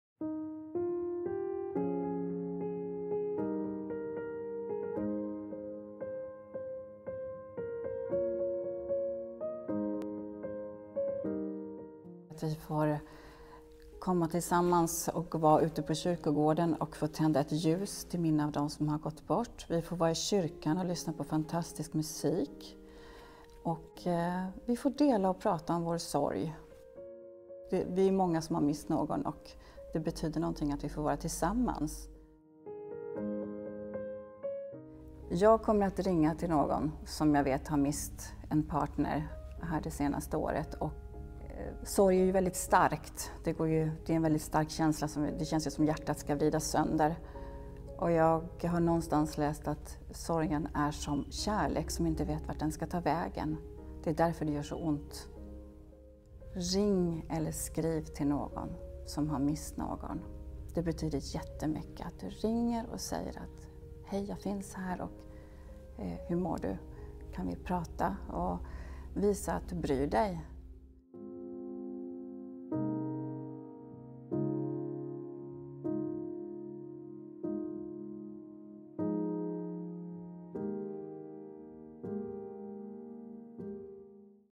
Hur möter man en människa i sorg? -Tankar inför Allhelgona
Vaktmästare Pia Romberg berättar om vad allhelgona betyder för henne och om hur man kan möta en människa i sorg.